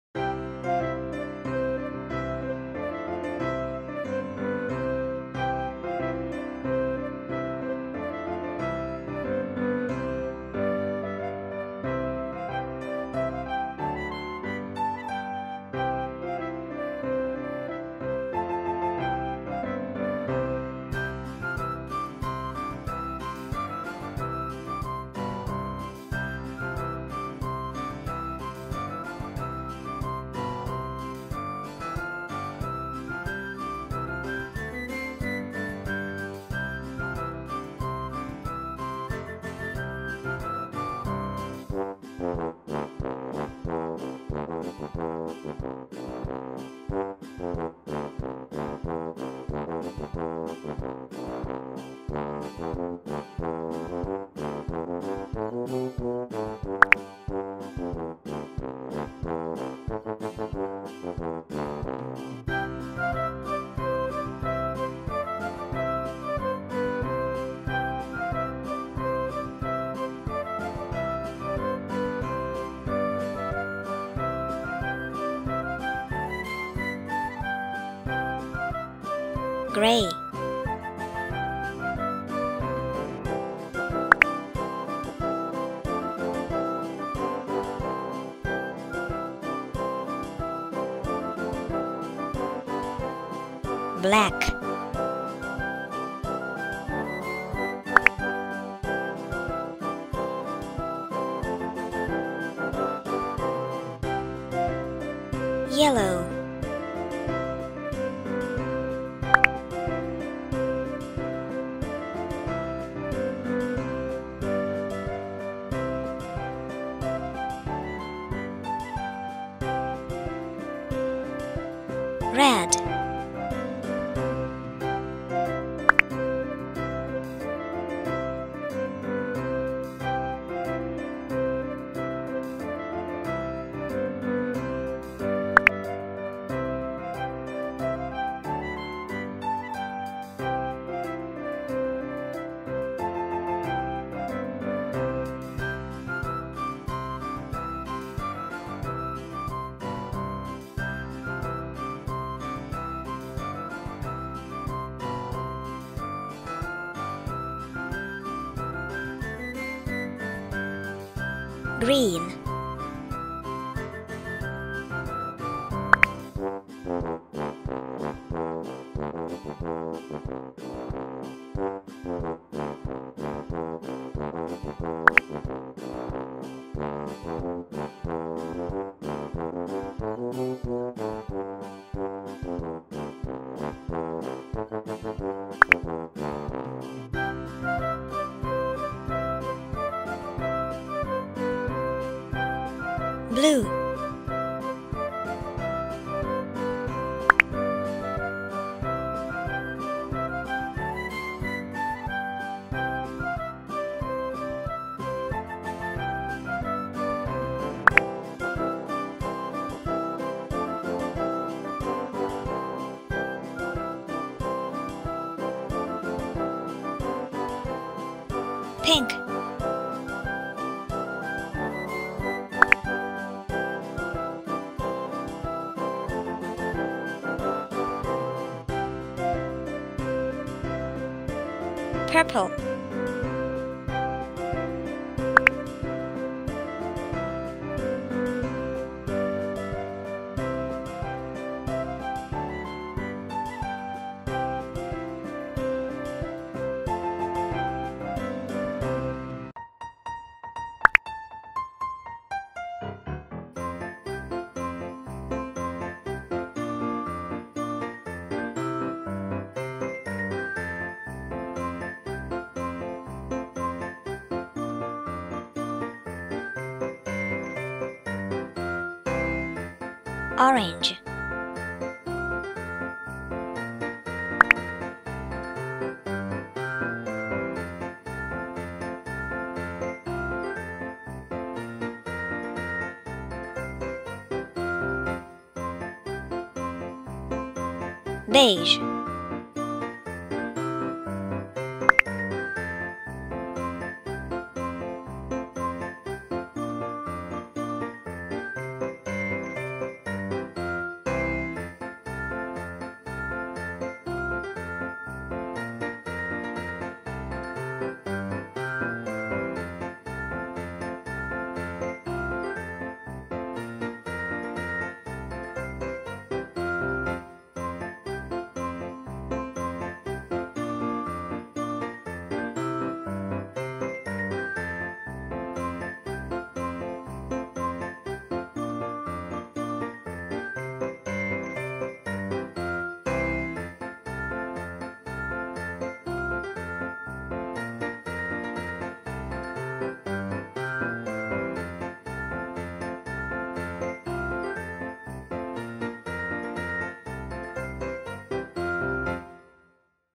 Christmas Room - Colouring Book for Toddlers
We painted a Christmas Tree, Presents, Stocking, Armchair, Wreath and more.